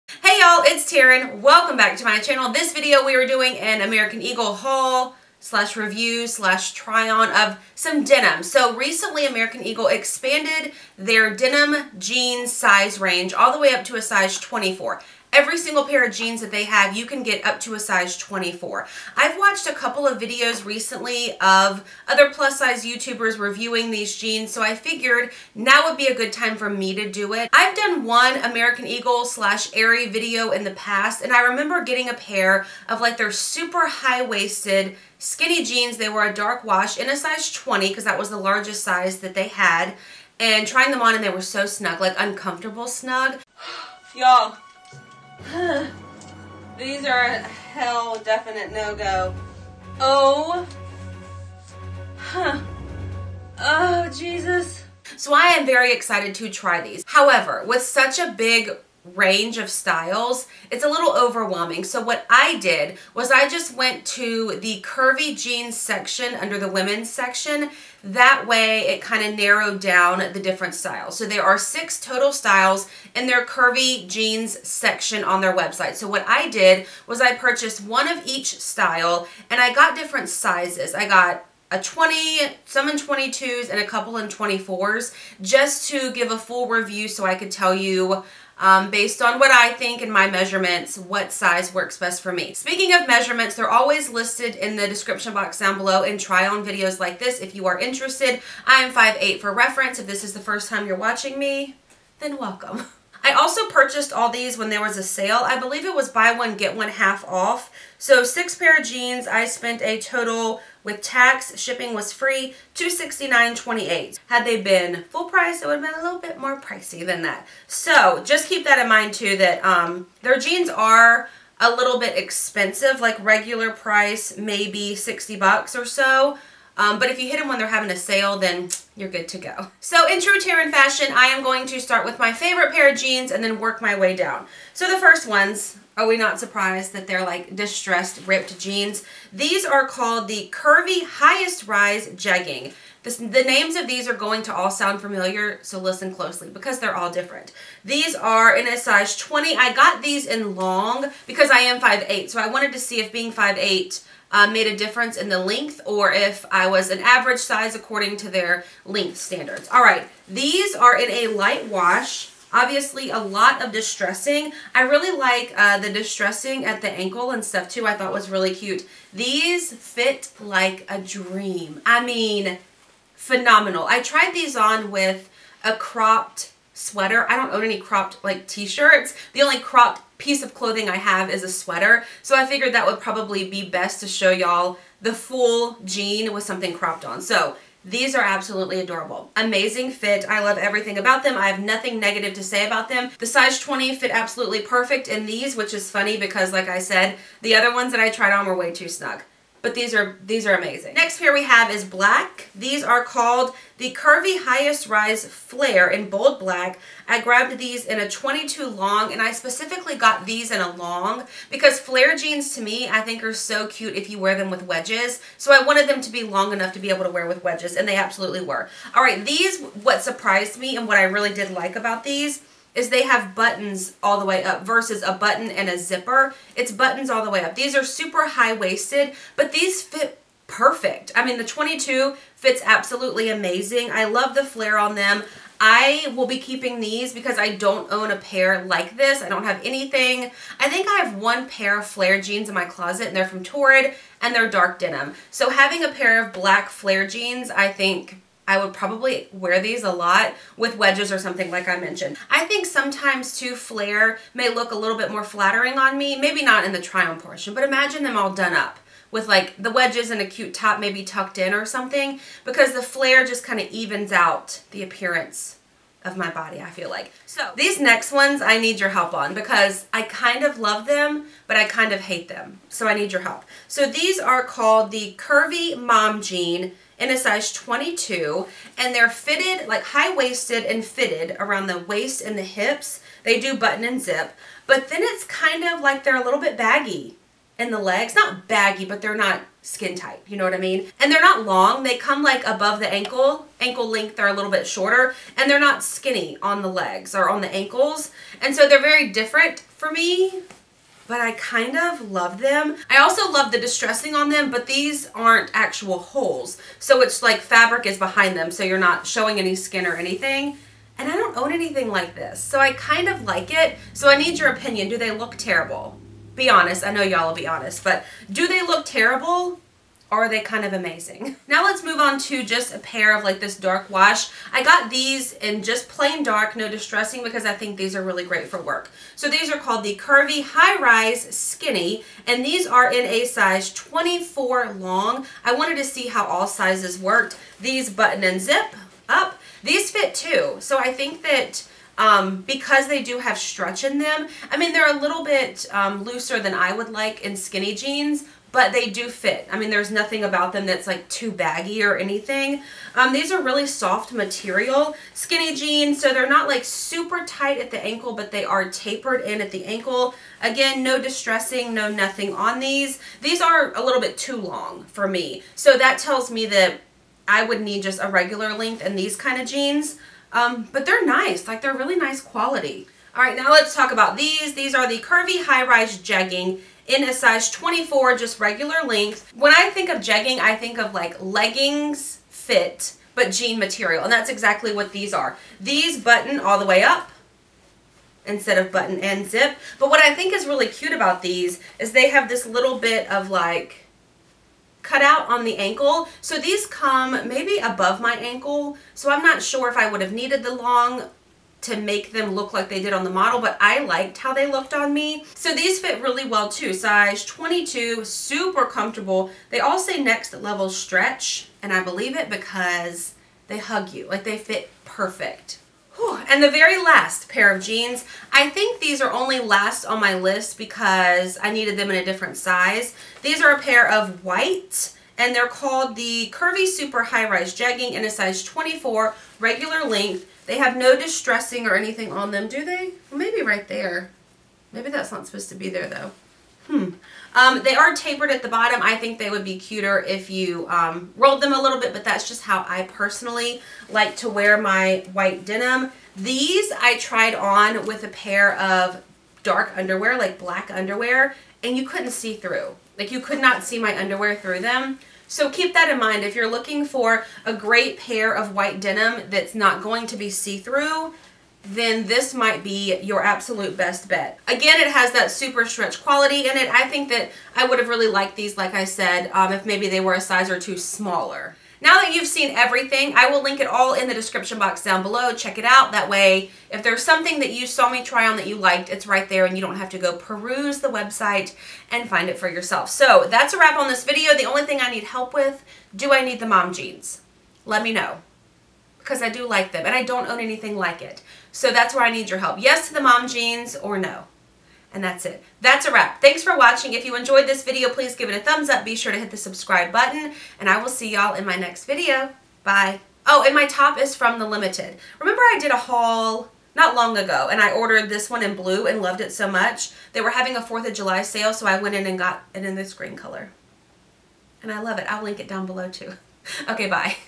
I TRIED ALL STYLES OF AMERICAN EAGLE CURVY JEANS | Taren Denise
Hey y'all! I wanted to try some denim again from American Eagle because they extended their plus size range :)

What I tried on:
Curvy highest rise jegging
Curvy hi-rise skinny
Curvy hi-rise jegging
Curvy highest rise flare (black)
Curvy super hi-rise jegging (white)
Curvy mom jean

I get asked a lot... so here are my measurements:
waist: 42"
hips: 56"
bra size: 38/40DD
Height: 5'8"